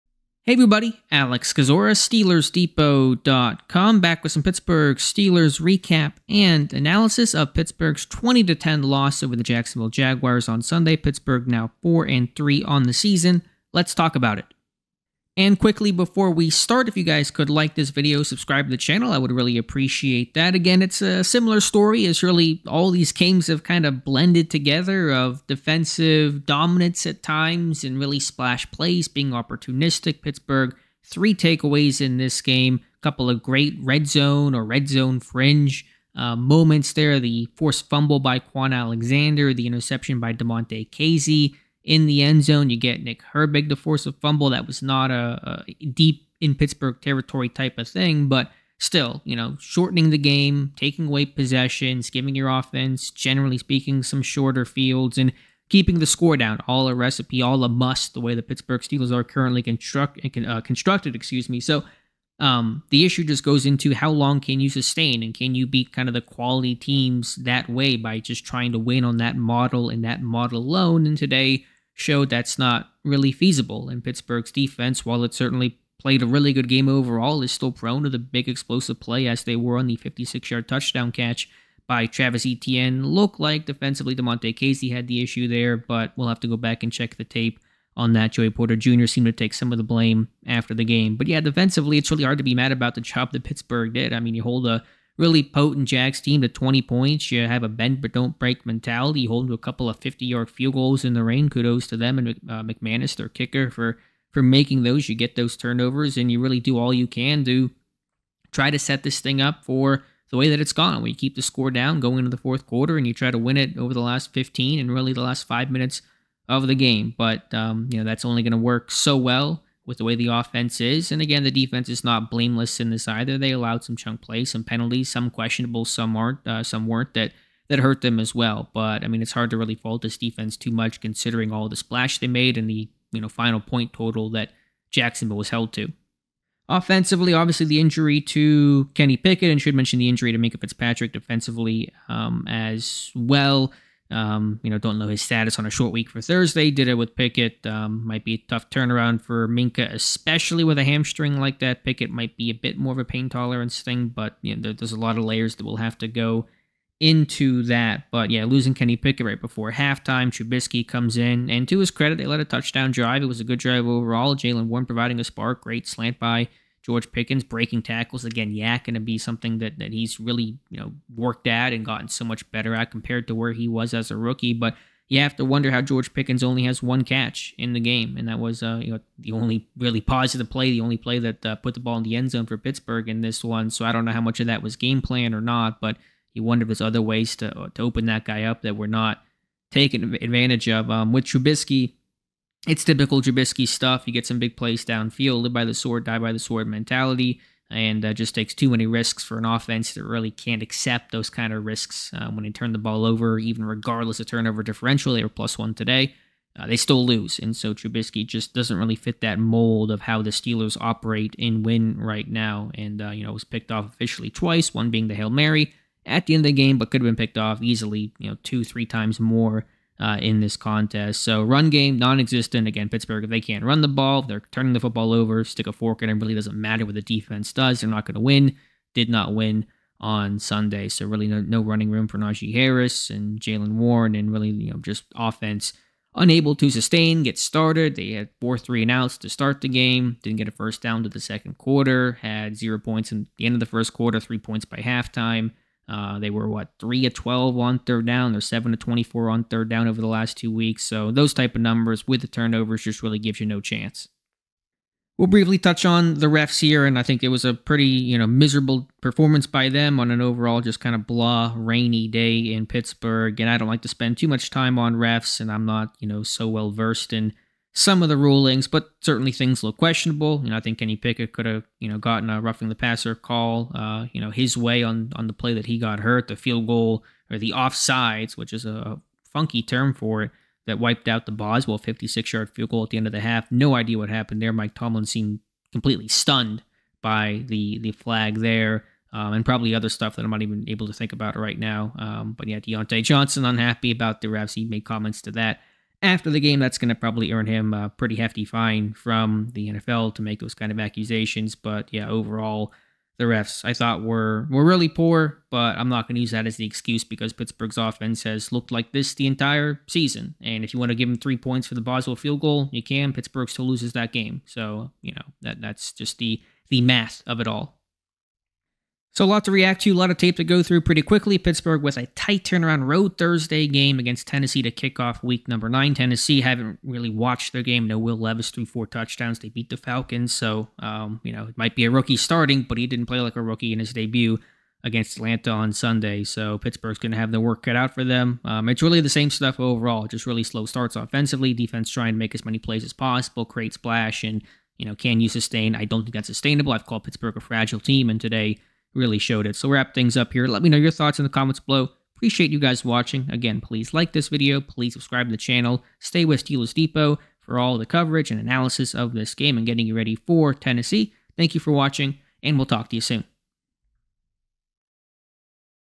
Steelers Vs. Jaguars Recap + Analysis
Breaking down the Pittsburgh Steelers Week 8 loss to the Jacksonville Jaguars.